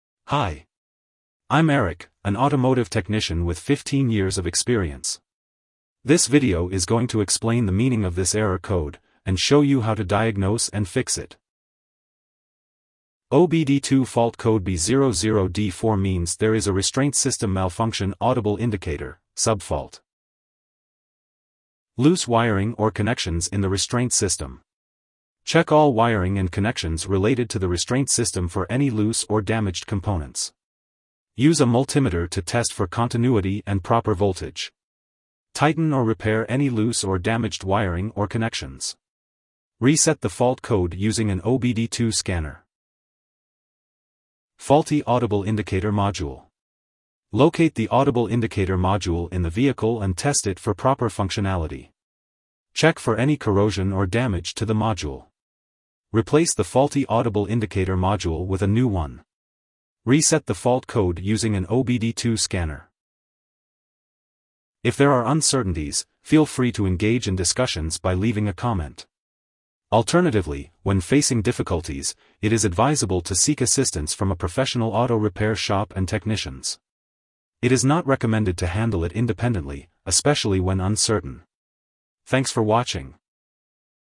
How to Diagnose and Fix B00D4 Engine Code - OBD II Trouble Code Explain
OBD-II fault code B00D4 means there is a Restraint System Malfunction Audible Indicator (Subfault)

8ad7